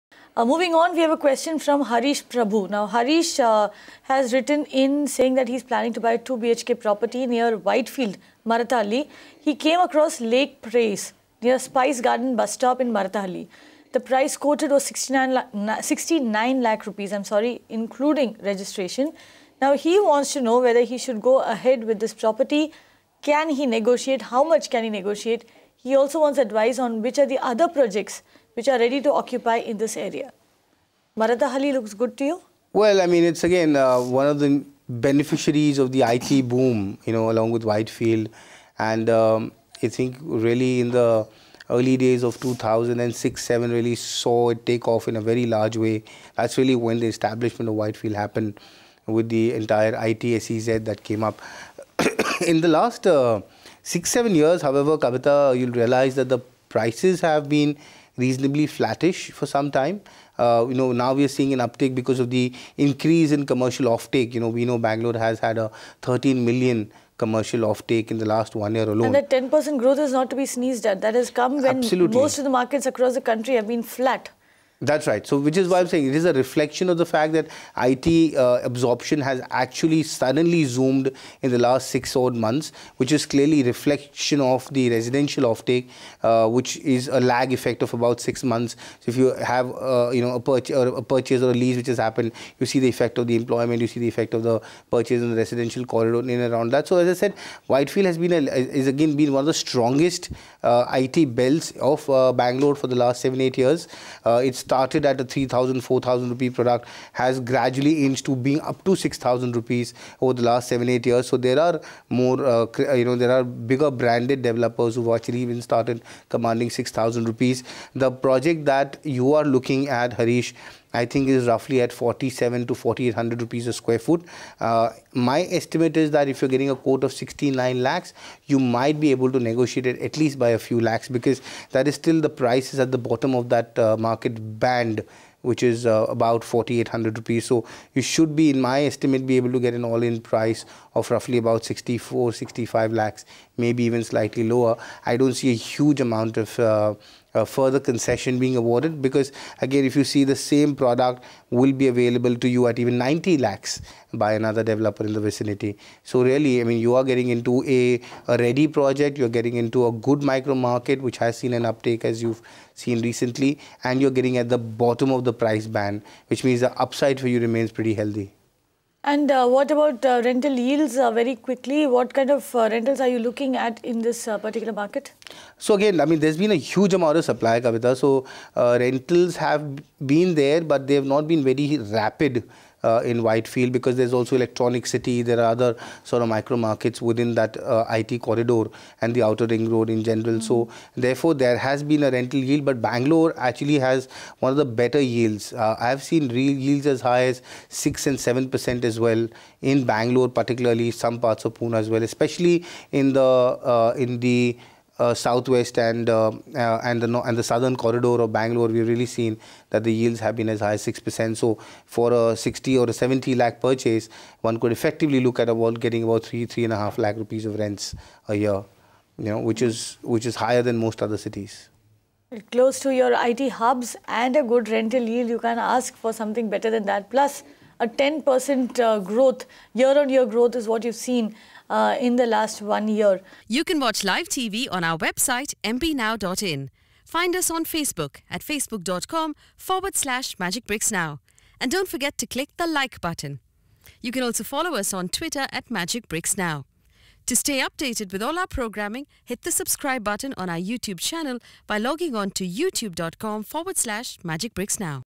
Would you recommend me to invest in Marathahalli?- Property Hotline
Expert: Amit Goenka, MD & CEO, Nisus Finance.

Question: I am planning to buy 2 BHK property near Whitefield / Marathahalli. I saw Lake Praise near Spice Garden bus stop Marathahalli. The price quoted is Rs 69 lakh including registration. Could you please advise whether to go ahead with this property and how much we can negotiate? Any advice on other projects that are ready to occupy?

Answer: In Marathahalli the average property rate is Rs 4190- Rs 6785 per sq. ft.  Average price of the property sized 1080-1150 sq. ft. ranges from Rs 58 – Rs 60 lakh. In FLUTE, 2 BHK flats sized 1309 sq. ft. are available. Average price in this project is Rs 63 lakh. Average price in Bren Unity is Rs 65 lakh. Purva Riviera will offer 2 BHK flat 1334 sq. ft. Average price in this project is Rs 90 lakh. You should invest at this point of time.